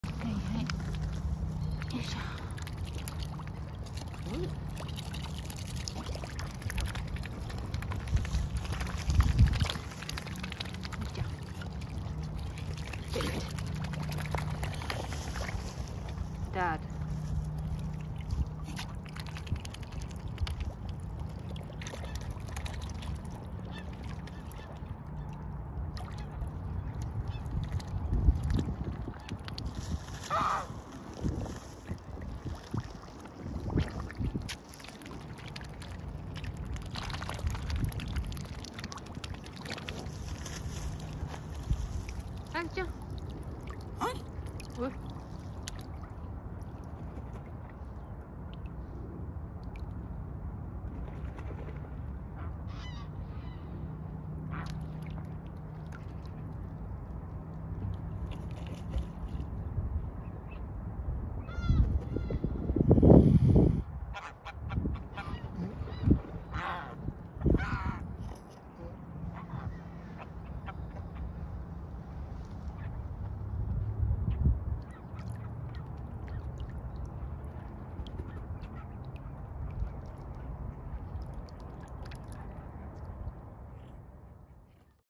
Dog alert 2
Kids reacted to Dad’s alert immediately. Good to be obedient.